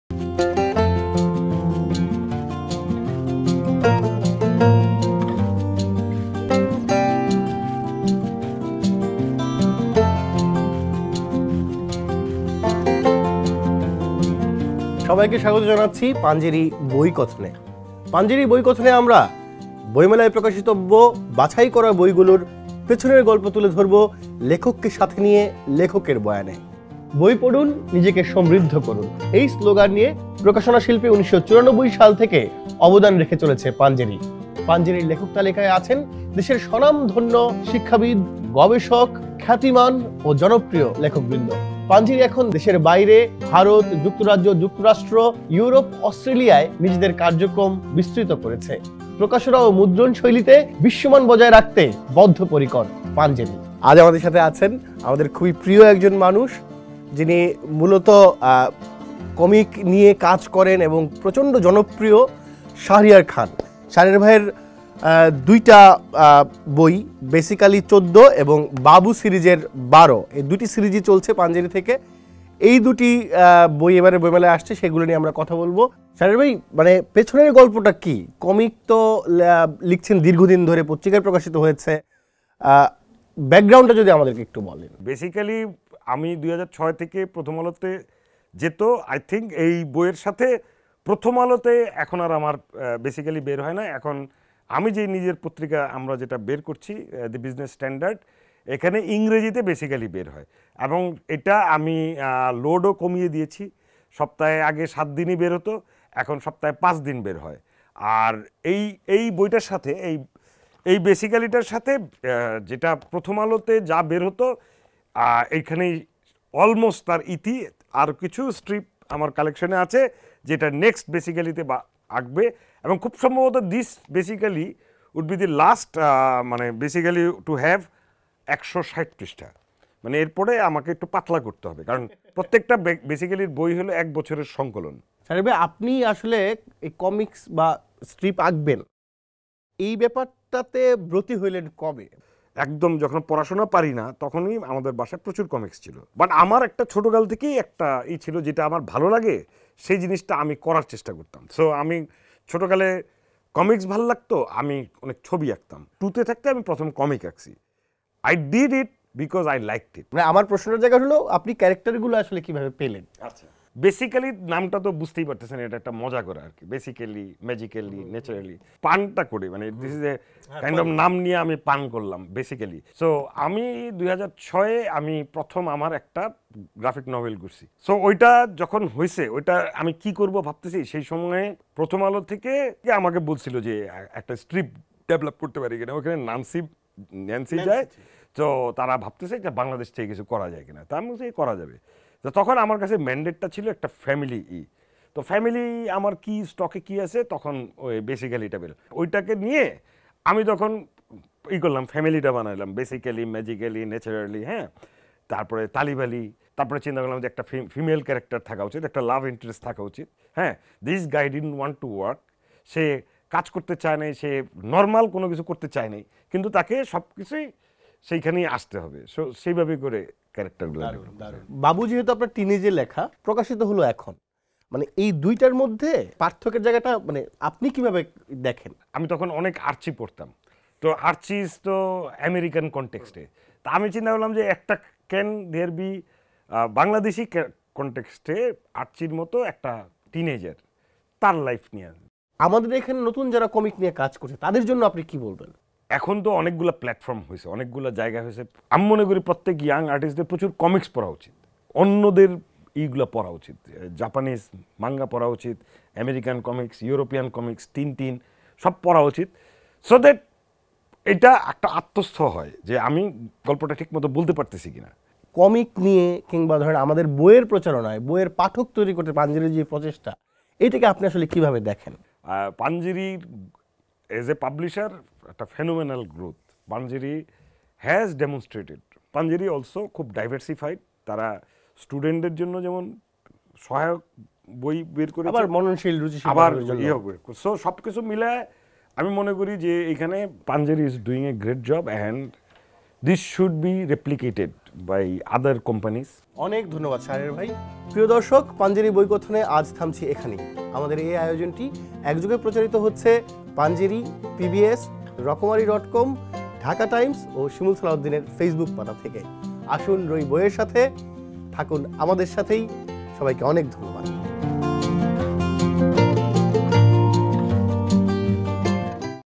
'পাঞ্জেরী বইকথন' এর দশম পর্বের অতিথি শাহরিয়ার খান
অমর একুশে বইমেলা উপলক্ষে পাঞ্জেরী অনবদ্য আয়োজন 'পাঞ্জেরী বইকথন'। এই আয়োজনের দশম পর্বের অতিথি জননন্দিত প্রখ্যাত কমিকশিল্পী ও ‘দ্য বিজনেস স্ট্যান্ডার্ড’-এর নির্বাহী সম্পাদক শাহরিয়ার খান। আসছেন পাঞ্জেরী থেকে তাঁর তুমুল  জনপ্রিয় কমিক্‌স সিরিজ ‘বেসিক আলী ১৪’ এবং ‘বাবু ১২’ নিয়ে।
শিমুল সালাউদ্দিনের সংগে এই আশ্চর্য সৃজনশীল কমিকশিল্পী শাহরিয়ার খানের জমাটি আড্ডায় জানবেন বইটি রচনার নেপথ্য কথা, অন্তর্গত কথা, তাঁর রচিত অন্য বই এবং প্রি-অর্ডার সম্পর্কেও। পর্যায়ক্রমে লেখক, পাঠক এবং প্রকাশকের মেলবন্ধনে পাঠাভ্যাস আন্দোলনকে বেগবান করে শক্তিশালী রিডিং সোসাইটি গড়ার অগ্রযাত্রায় আপনিও আমাদের সংগী হোন।

‘বেসিক আলী ১৪’ এবং ‘বাবু ১২’ বই দু’টি অনলাইনে অর্ডার করতে ভিজিট করুন: